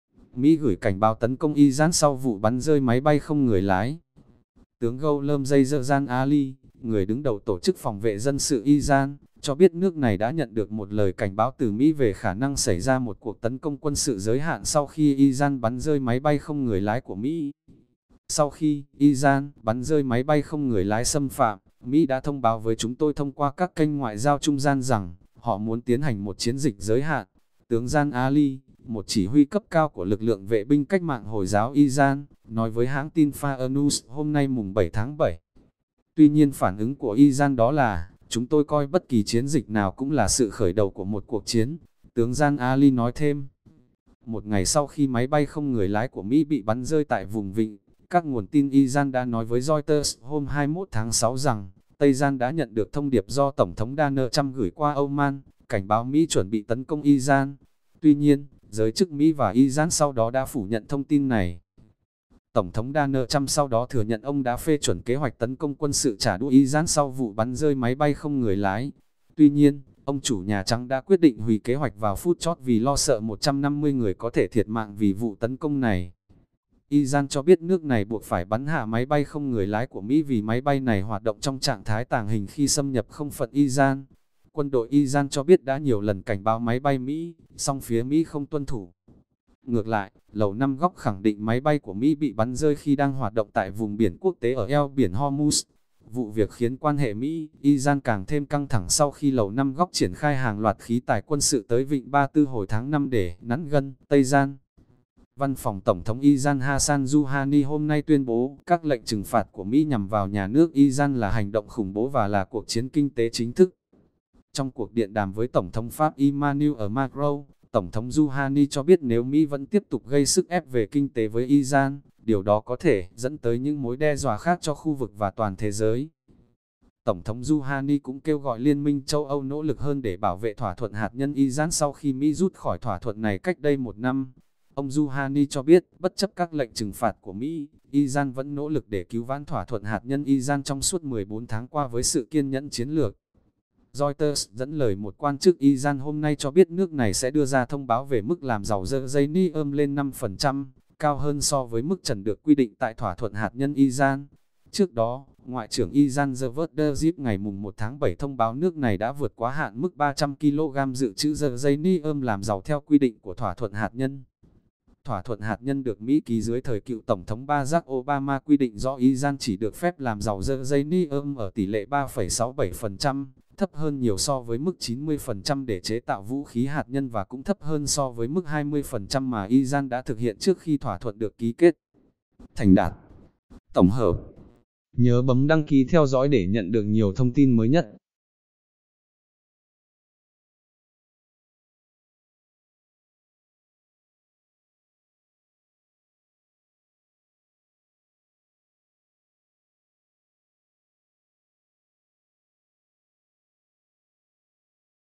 Mỹ gửi cảnh báo tấn công Iran sau vụ bắn rơi máy bay không người lái
(Dân trí) - Mỹ đã gửi thông điệp cảnh báo Iran về một cuộc tấn công sau khi Tehran bắn rơi máy bay không người lái của Washington tại vùng Vịnh.